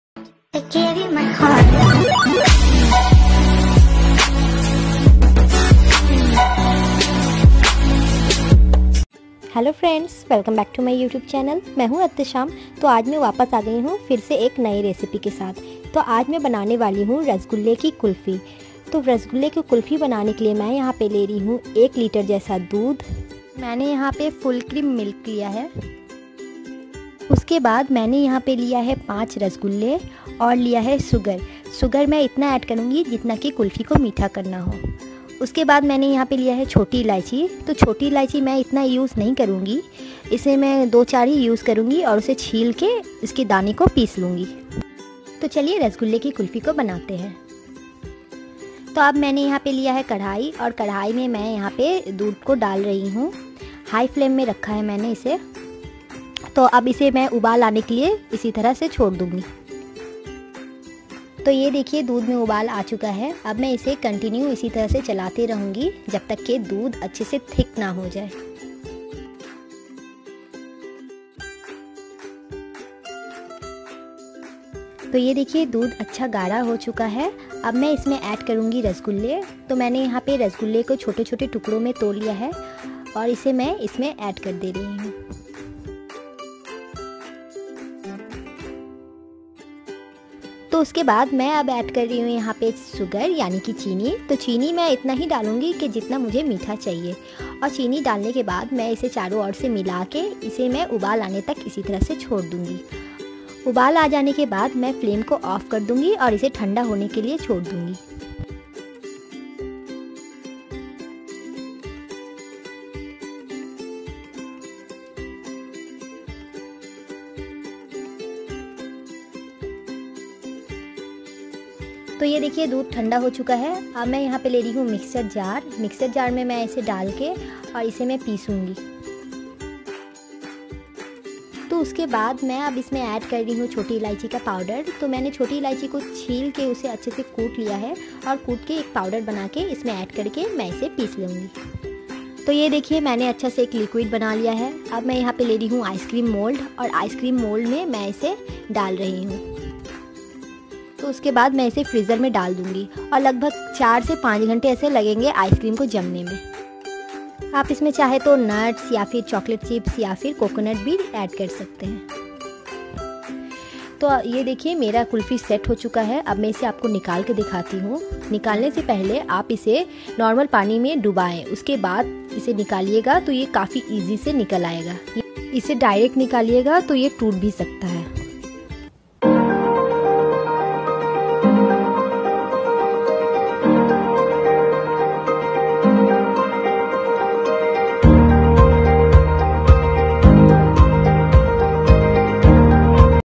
Rasgulle ki kulfi (Popsicle) | রসগোল্লার কুলফি বানান
Hello guys, kaise hain aaplog?  Welcome to WestBengal Beauty ,main Attesham laut aayi aaj fir ek cooking video lekar jisme main dikhane jarhi hu ke Kulfi kaise bnate hai wo v Rasgulle ki, jii haan friends rasgulle ki kulfi bnayenge aaj hum milkar in simple steps!!! Toh video ko last tak zarur dekhe!!
Shukriya!!!

I hope you all will like the video!
If you loved watching  this video then,

Thanks!!! 🥰